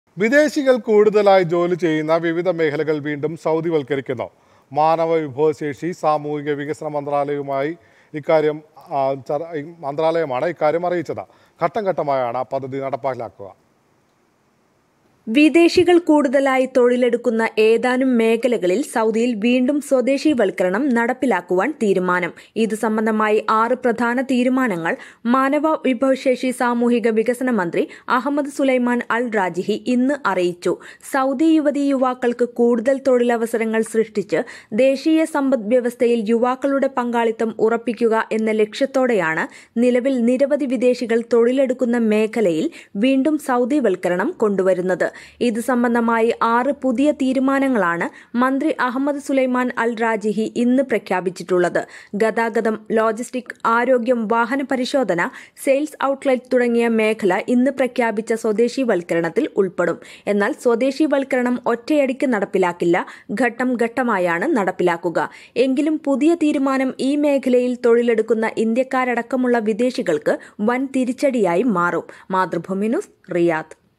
വിദേശികള്‍ കുടുതലായി ജോലിചെയ്യുന്ന വിവിധ മേഖലകള്‍ വീണ്ടും സൗദിവത്കരിക്കുന്നു | Mathrubhumi News
വിദേശികള്‍ കുടുതലായി ജോലിചെയ്യുന്ന വിവിധ മേഖലകള്‍ വീണ്ടും സൗദിവത്കരിക്കുന്നു. മാനവ വിഭവശേഷി, സാമൂഹിക വികസന മന്ത്രാലയമായി ഇക്കാര്യം അറിയിച്ചത്. ഘട്ടംഘട്ടമായാണ് പദ്ധതി നടപ്പിലാക്കുക.

Happy viewing!